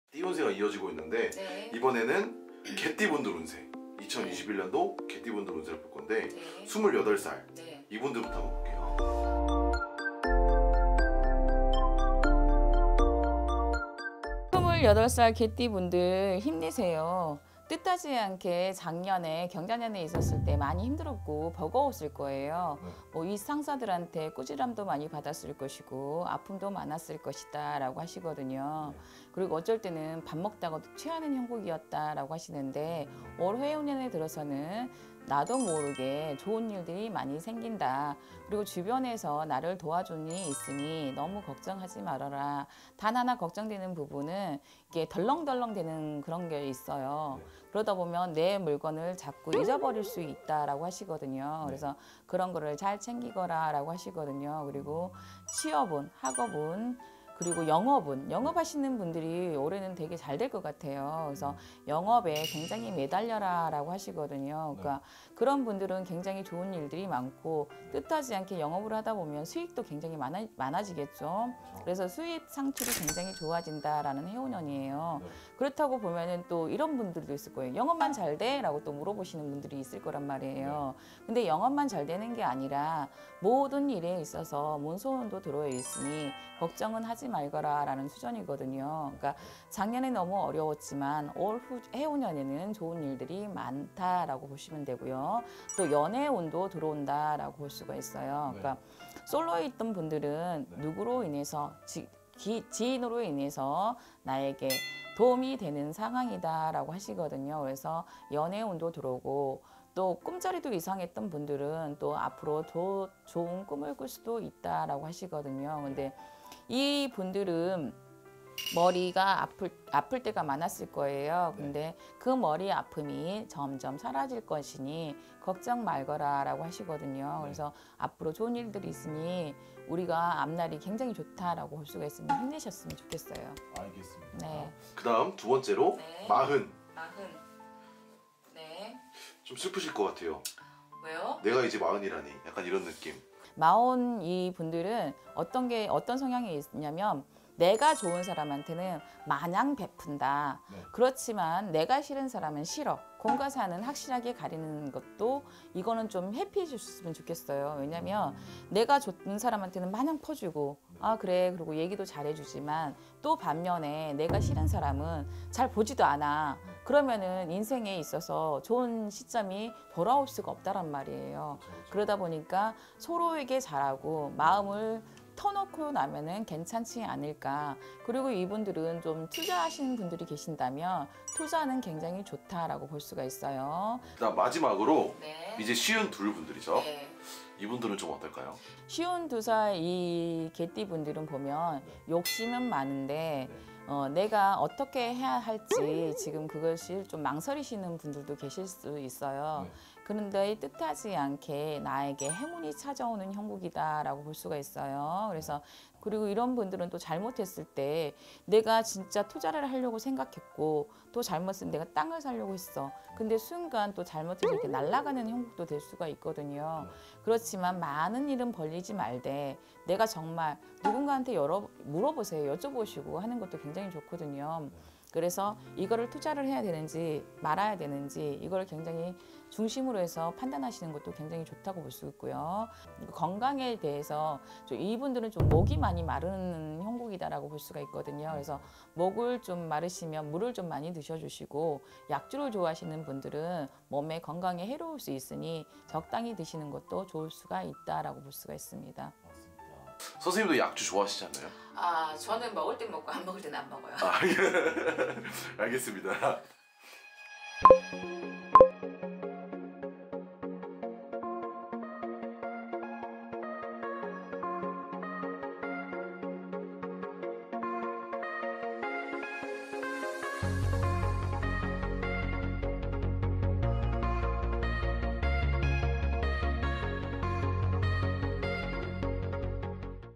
​[2021년 운세] 신축년 개띠 운세?! '개띠분들 XX때문에 힘들수도,...!!' (28세 40세 52세 64세 76세 개띠 운세) [부천점집 일월성신]
자막 : 나눔템플릿 2021년대박나는띠​ 2021년개띠운세​

★대표블로그★
-올마이티TV